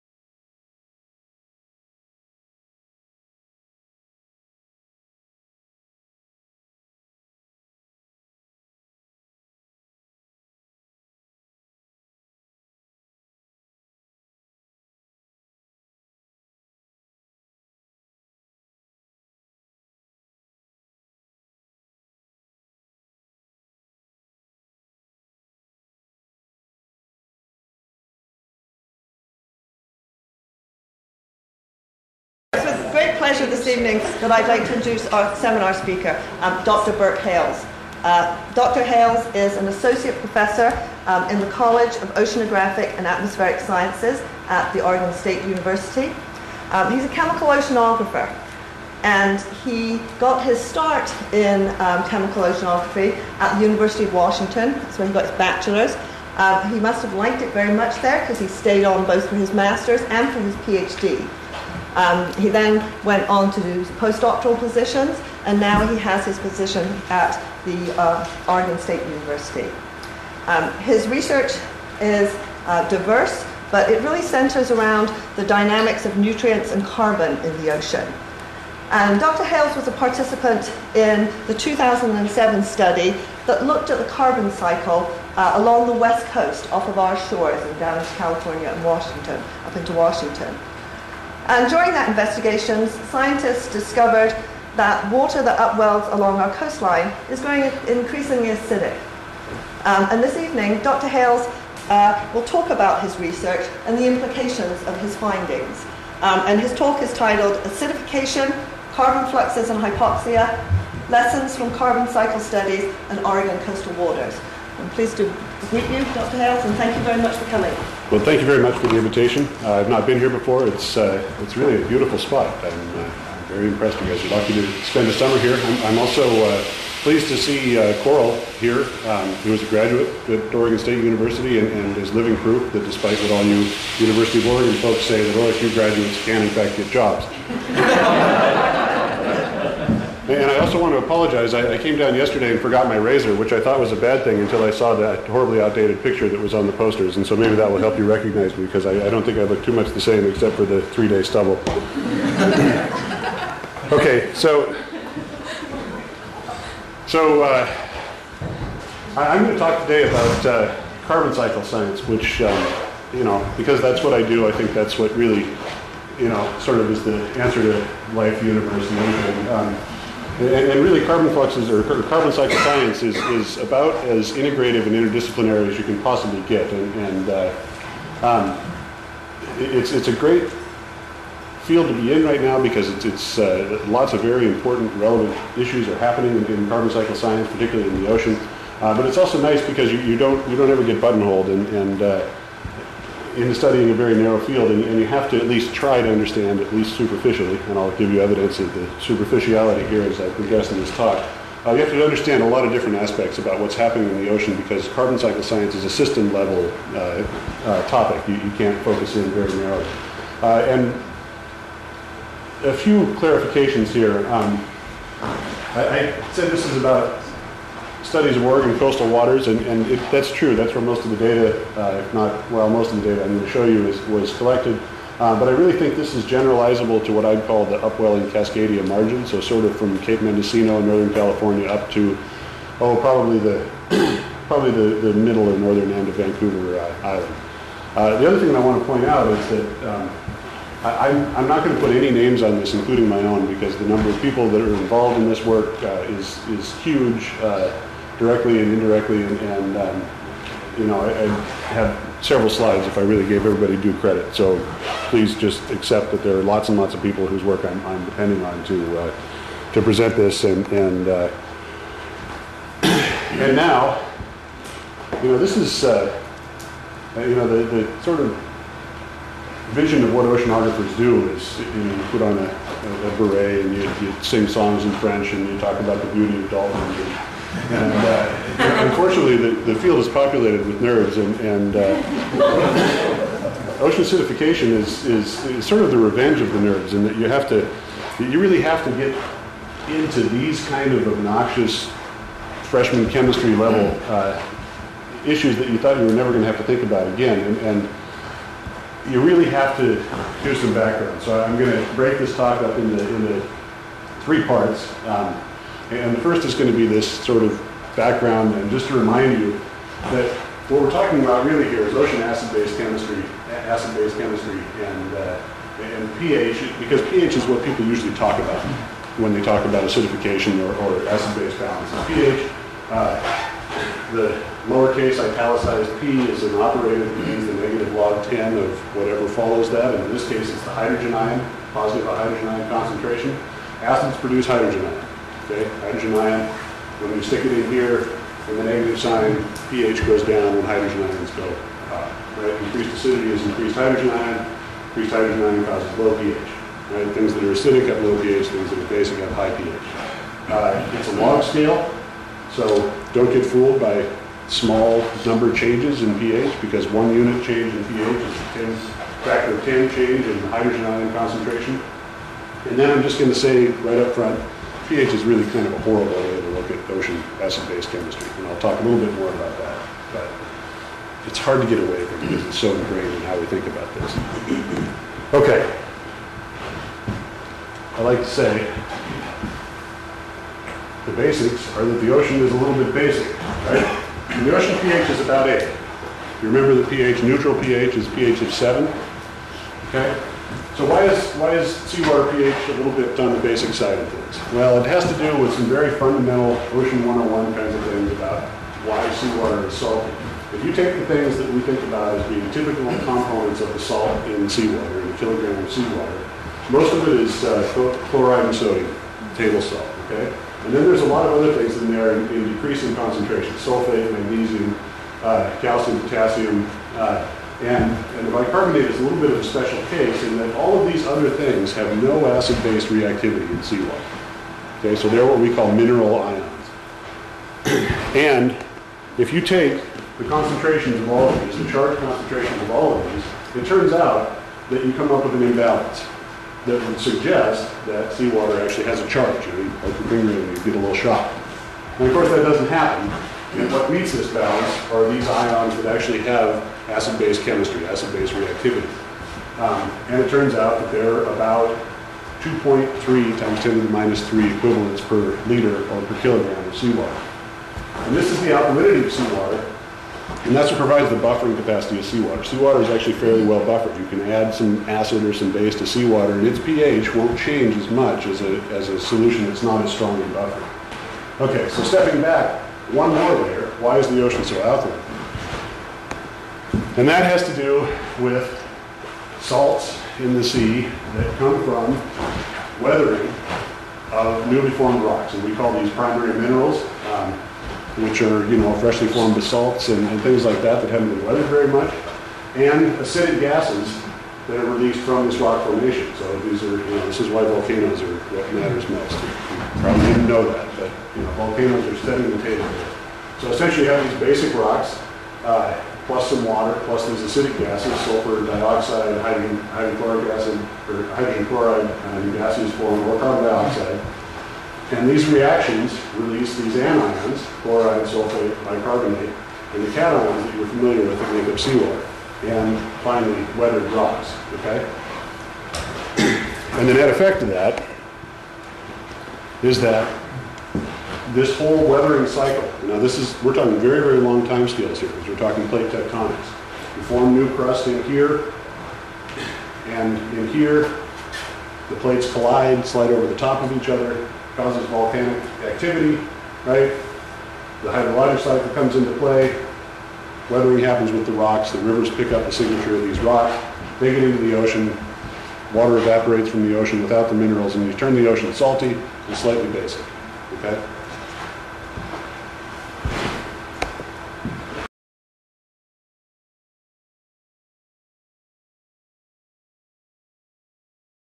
Ocean Acidification Part 1 of 7
How carbon in the atmosphere is changing the oceans and altering sea life within them. Featuring Prof. Burke Hales, Oregon State University, speaking at the Oregon Institute of Marine Biology Summer Lecture Series